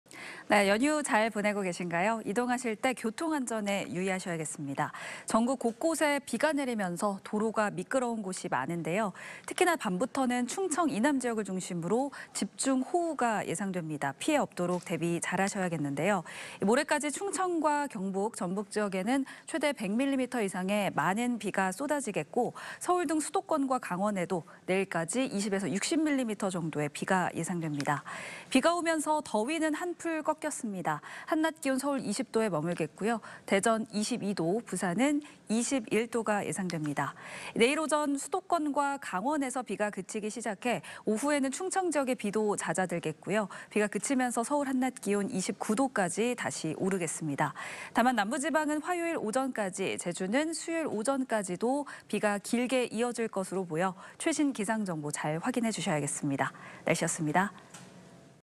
[날씨] 전국 흐리고 비…밤부터 충청 이남 집중호우
연휴 잘 보내고 계신가요? 이동하실 때 교통안전에 유의하셔야겠습니다.

전국 곳곳에 비가 내리면서 도로가 미끄러운 곳이 많은데요.

특히나 밤부터는 충청 이남 지역을 중심으로 집중호우가 예상됩니다. 피해 없도록 대비 잘하셔야겠는데요.

모레까지 충청과 경북, 전북 지역에 최대 100mm이상의 많은 비가 내리겠고 서울 등 수도권과 강원에도 내일까지 20-60mm정도의 비가 예상됩니다.

비가 내리면서 더위는 한풀 꺾였는데요, 한낮 기온 서울 20도에 그치겠고 대전 22도, 부산은 21도가 예상됩니다.

내일 오전, 수도권과 강원에서 비가 그치기 시작해 오후에는 충청 지역의 비도 잦아들겠고요. 비가 그치면서 서울 한낮 기온 29도까지 오르겠습니다.

다만 남부지방은 화요일 오전까지, 제주는 수요일 오전까지도 비가 길게 이어질 것으로 보여 최신기상정보, 잘 확인해주셔야겠습니다. 날씨였습니다.